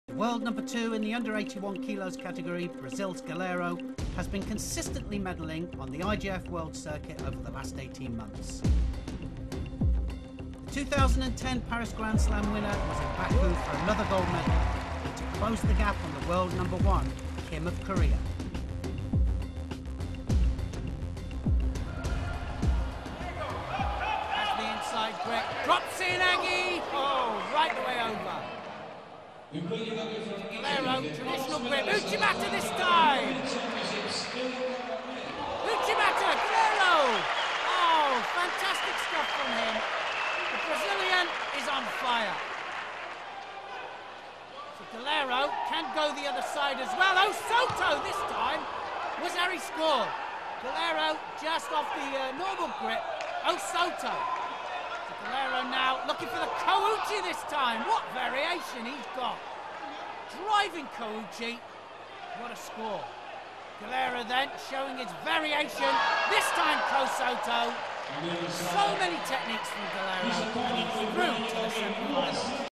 Leandro Guilheiro Throwing Down.
A real slam fest from an excellent Brazilian Judoka.